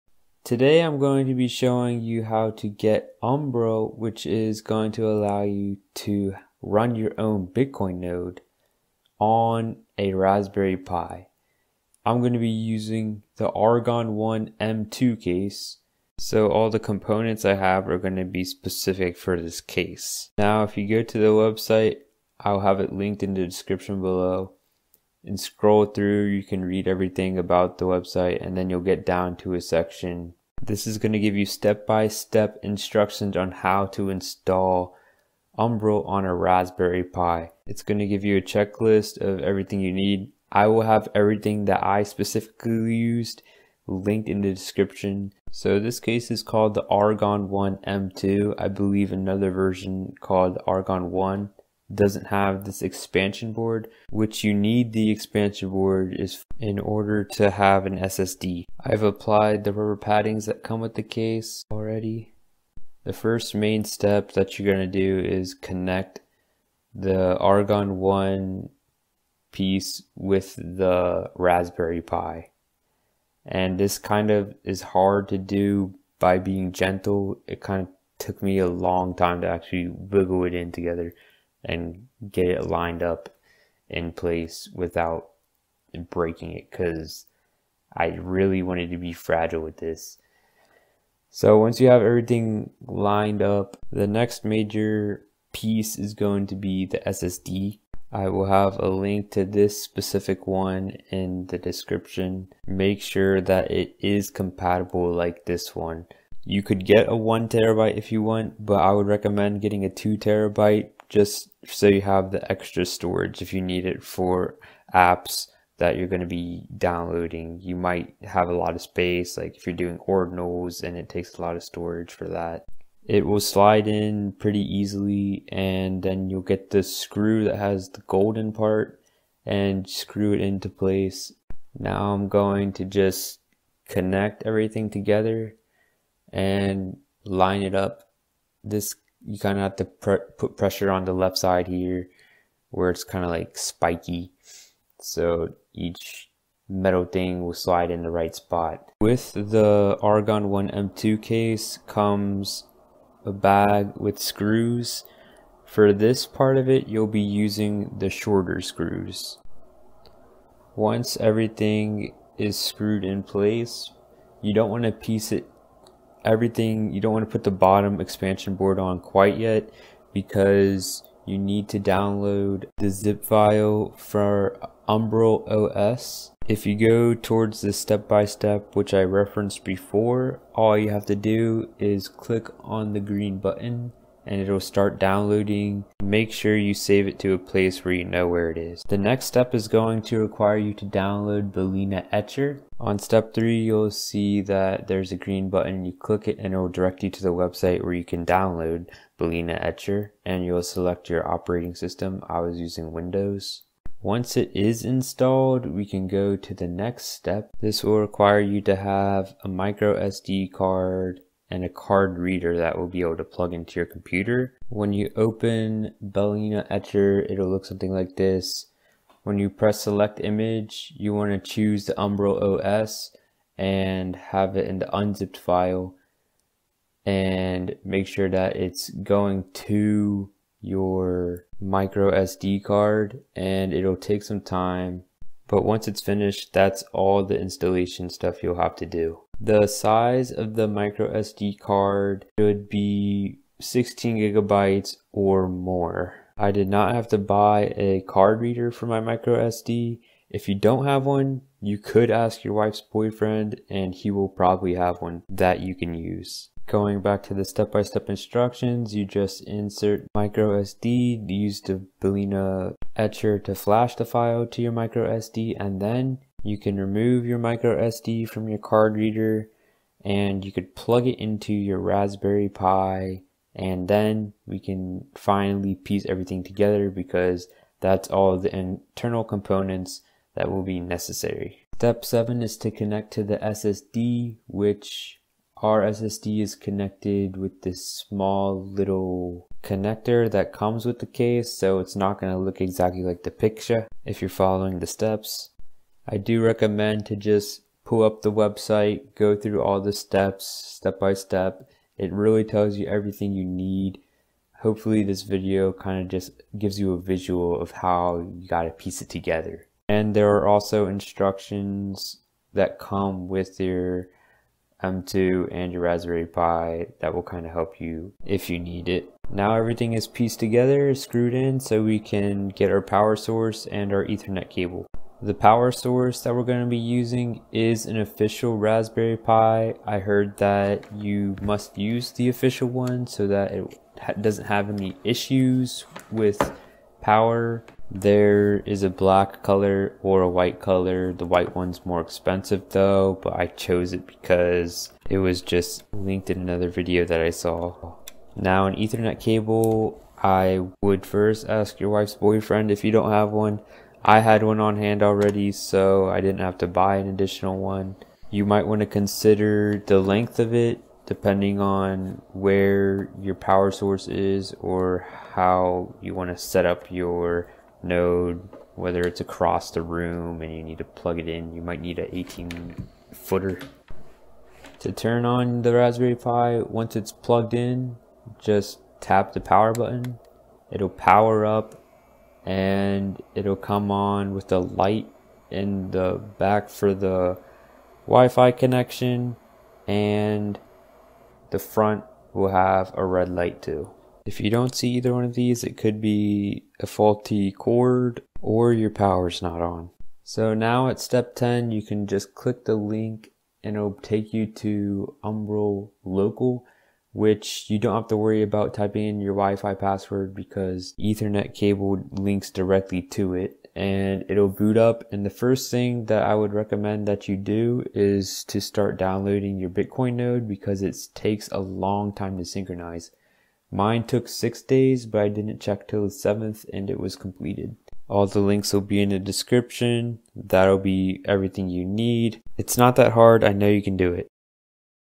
Build a Bitcoin Node with Argon One M.2 Case for Raspberry Pi 4
(MicroSD Card 16GB)

(Raspberry Pi 4 Power Supply)

(M.2 SATA SSD 2280 2tb) Note:You can search argon one in the reviews on amazon and other buyers have said it works.

(Argon One M2)

(Raspberry Pi 4)

You'll also need a card reader for the micro SD and an ethernet cable to link directly to your internet.